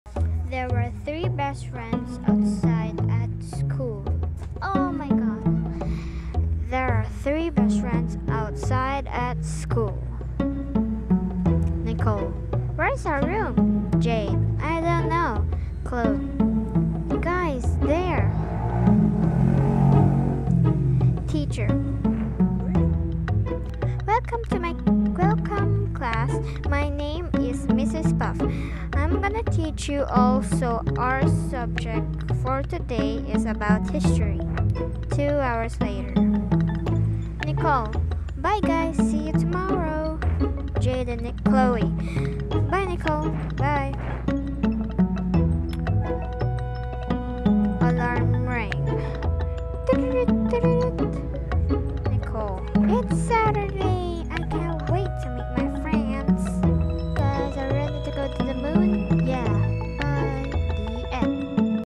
The 3 BestFriendZ
Nicole,Jade and Chloe are ready to go to school so Enjoy watching!